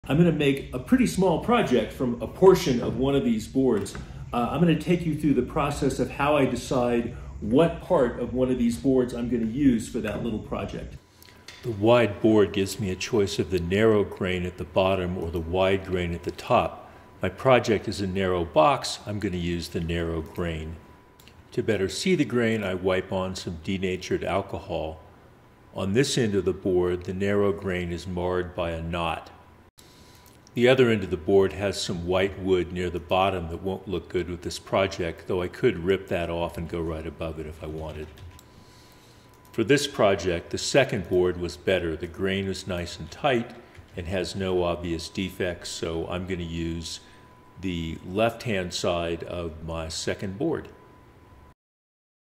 Small Project, Big Board
I have a couple beautiful WIDE cherry boards -- need to pick the best part for a small project.